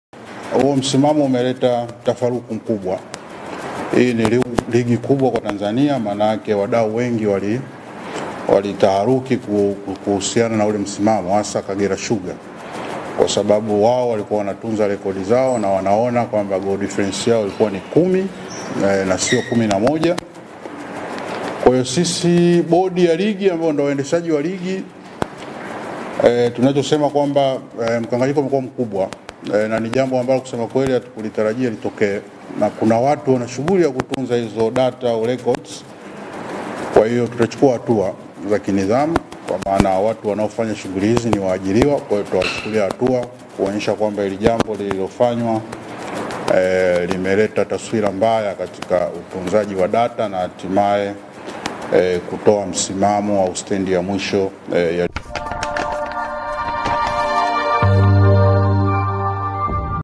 Bodi ya Ligi wachemka, Kagera yarudishwa Ligi Kuu
Bodi ya Ligi Tanzania (TPLB) imetangaza kufanya marekebisho katika msimamo wa Ligi Kuu Bara huku ikisema itachukua hatua kwa watunza takwimu wake waliopotosha.

Awali ilionekana Kagera Sugar ndio imeteremka lakini Mkurugenzi wa Bodi ya Ligi, Boniface Wambura amesema marekebisho waliyofanya Stand United inashuka na nafasi ya juu inakwenda kwa Kagera Sugar.

Maaana yake Stand United ndio iliyoteremka daraja na nafasi ya kucheza Play Off inakwenda kwa Kagera Sugar.

Kagera Sugar itaivaa Pamba Juni 2 katika mechi ya Play Off na mshindi atacheza Ligi Kuu Bara na Mwadui FC itamenyana na Geita.

Baada ya ligi kumalizika jana, kumekuwa na gumzo na kizumgumkuti huku watu wakiwa hawajui timu iliyoteremka kwa kuwa tawimu za Gazeti la Championi na Azam Tv zilionyesha aliyeshuka ni Stand United huku takwimu za bodi ya ligi zilionyesha ni Kagera Sugar ndio iliyoteremka.